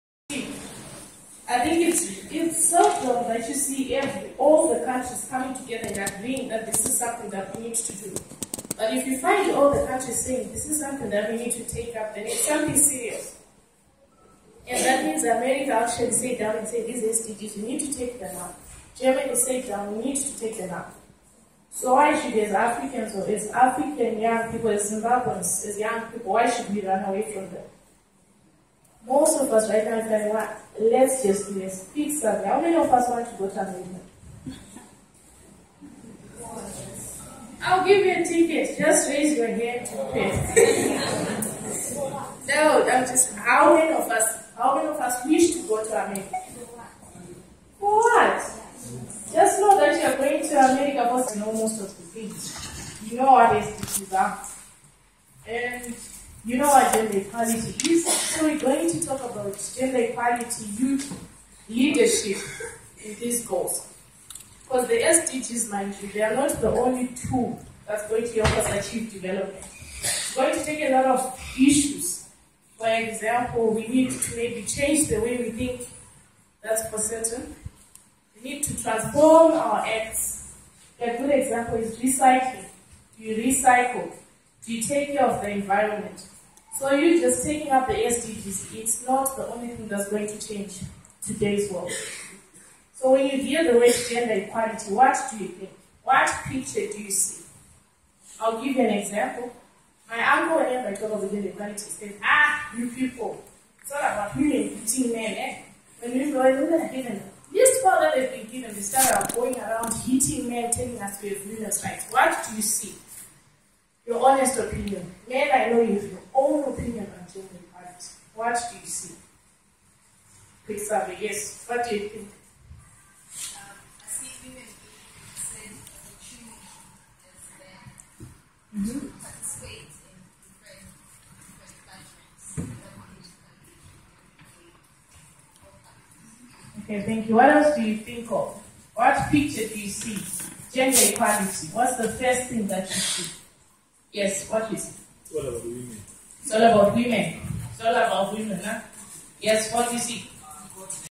Hlengisile Jiyane on at GZU SDG Lecture 2017
Great Zimbabwe University ZUNA Chapter hosted its SDG Lecture at School of Law Campus in Masvingo recently. Miss Hlengisile Jiyane was the Guest Speaker who touched mainly on SDG#5 Gender Equality.